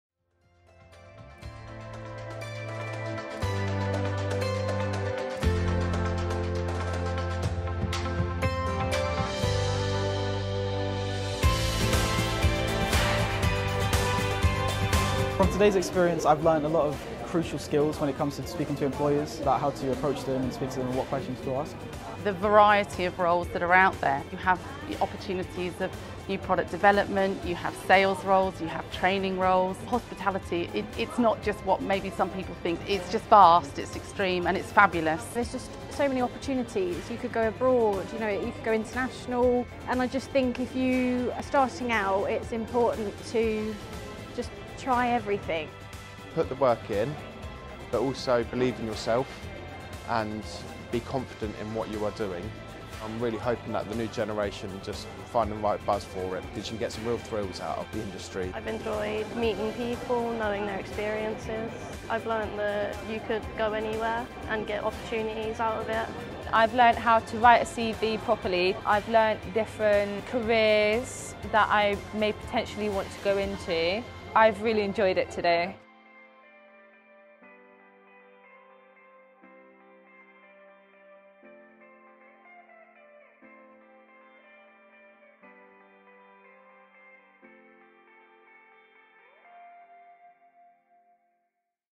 Talent Showcase Workshop matches students with businesses
More than 60 students joined the latest Talent Showcase Workshop to meet potential employers in the tourism and hospitality sector.

The event provided students searching for jobs with the opportunity to meet businesses, learn what opportunities are available and share what they have to offer, whilst also helping them develop their interview skills.